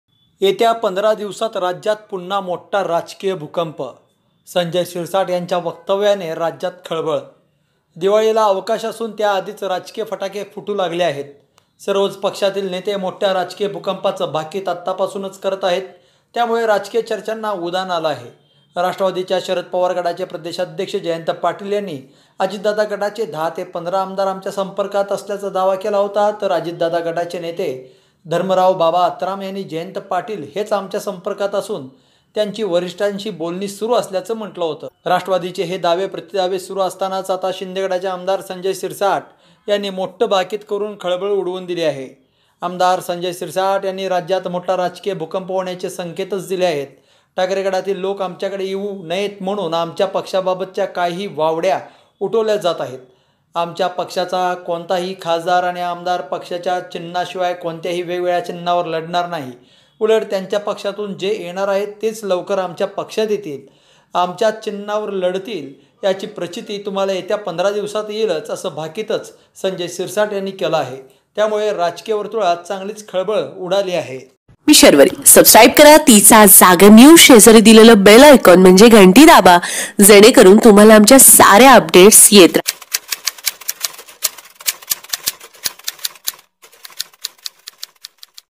येत्या 15 दिवसात राज्यात पुन्हा मोठा राजकीय भूकंप?/Sanjay Shirsat/Jayant Patil/Eknath Shinde/NCP/BJP
teechajaagarnews#maharashtra
आम्हाला फॉलो करा तुमचं* com/Sharvaripawarnewseditor/